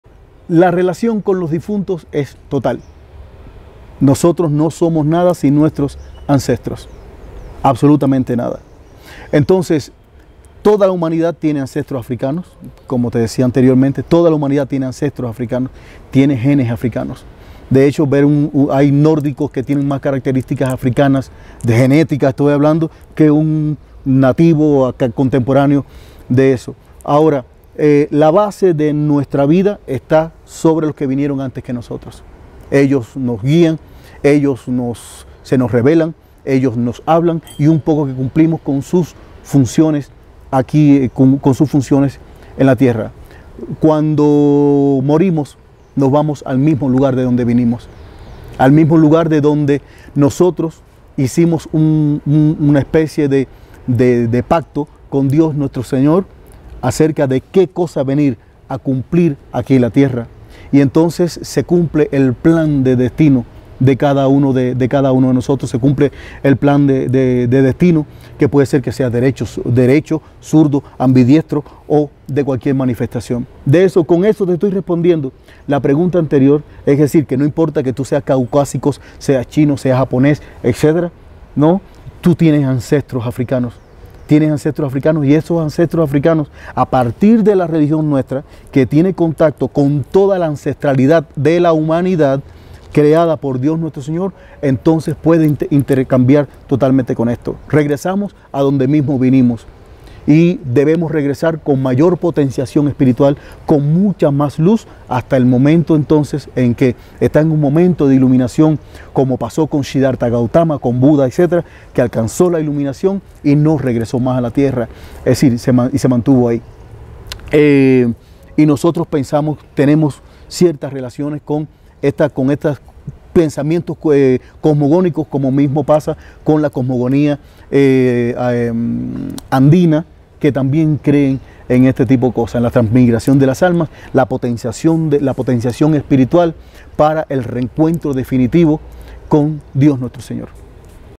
93 LA RELACION CON NUESTROS DIFUNTOS ES PERMAMENTE EN LAS PRACTICAS AFROCUBANAS
Juan Lázara debate con importantes especialistas sobre patrimonio funerario los vínculos entre las tradiciones europeas y americanas en Bolivia que derivaron en los más variados sincretismo. Las entrevistas se realizaron  durante el XIX Encuentro Iberoamericano de Valoración y Gestión de Cementerios Patrimoniales celebrado en Cochabamba, Bolivia en noviembre de 2018.